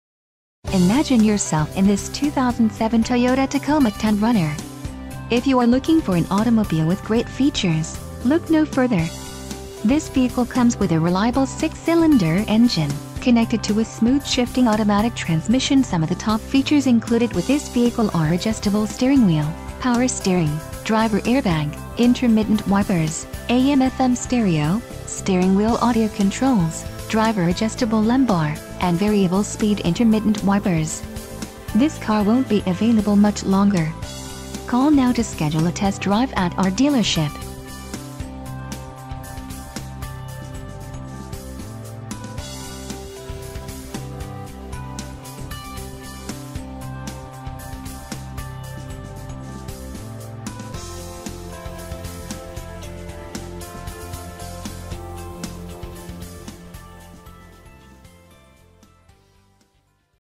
2007 Toyota Tacoma - Chapman Chevrolet - Tempe, AZ 85283
Imagine yourself in this 2007 Toyota Tacoma X-Runner. If you are looking for an automobile with great features, look no further. This vehicle comes with a reliable 6 cylinder engine, connected to a smooth shifting automatic transmission.. Some of the top features included with this vehicle are Adjustable Steering Wheel, Power Steering, Driver Air Bag, Intermittent Wipers, AM/FM Stereo, Steering Wheel Audio Controls, Driver Adjustable Lumbar, and Variable Speed Intermittent Wipers. This car won't be available much longer.